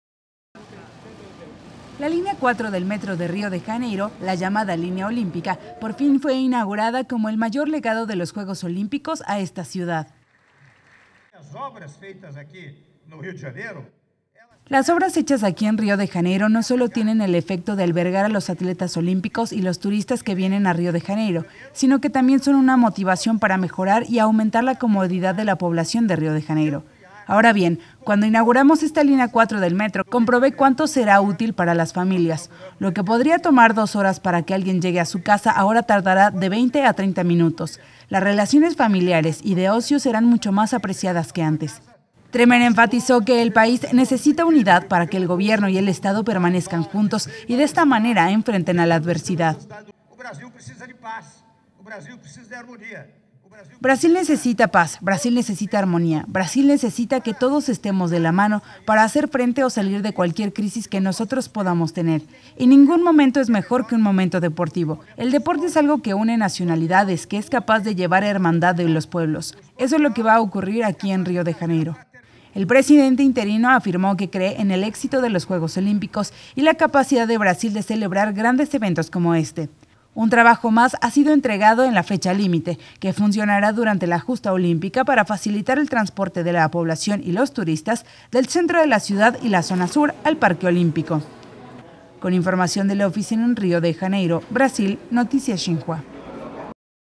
Inauguran línea olímpica del metro de Río de Janeiro
BRASIL-JJOO-SOCIEDAD-LINEA OLIMPICA
Intro: La línea 4 del metro de Río de Janeiro, la llamada "línea olímpica", por fin fue inaugurada, como el mayor legado de los Juegos Olímpicos a esta ciudad
Fecha: 30/07/2016
Locación: Río de Janeiro, Brasil
Duración: 00:01:37
Formato: MPEG 2 / HD 16:9
Idioma: Español
Sonido: Natural / Soundbite: portugués
Fuente: Xinhua
Sin restricciones

STORYLINE
Voz off
La línea 4 del metro de Río de Janeiro, la llamada "línea olímpica", por fin fue inaugurada, como el mayor legado de los Juegos Olímpicos a esta ciudad.

(SOUNDBITE, portugués) - Michel Temer, Presidente interino de Brasil
"Las obras hechas aquí en Río de Janeiro, no sólo tienen el efecto de albergar a los atletas olímpicos y los turistas que vienen a Río de Janeiro, sino que también son una motivación para mejorar y aumentar la comodidad de la población de Río de Janeiro. Ahora bien, cuando inauguramos esta línea 4 del metro, comprobé cuánto será útil para las familias. Lo que podría tomar dos horas para que alguien llegue a su casa, ahora tardará de 20 a 30 minutos. Las relaciones familiares y de ocio, serán mucho más apreciadas que antes".

Voz off
Temer enfatizó que el país necesita unidad, para que el gobierno y el estado permanezcan juntos y de esta manera enfrenten a la adversidad.

"Brasil necesita paz, Brasil necesita armonía, Brasil necesita que todos estemos de la mano, para hacer frente o salir de cualquier crisis que nosotros podamos tener. Y ningún momento es mejor que un momento deportivo. El deporte es algo que une nacionalidades, que es capaz de llevar hermandad de los pueblos. Eso es lo que va a ocurrir aquí en Río de Janeiro".

Voz off
El presidente interino afirmó que cree en el éxito de los Juegos Olímpicos y la capacidad de Brasil de celebrar grandes eventos como este.

Un trabajo más ha sido entregado en la fecha límite, que funcionará durante la justa olímpica, para facilitar el transporte de la población y los turistas del centro de la ciudad y la Zona Sur, al Parque Olímpico.

Con información de la oficina en Río de Janeiro, Brasil, Noticias Xinhua. (DL)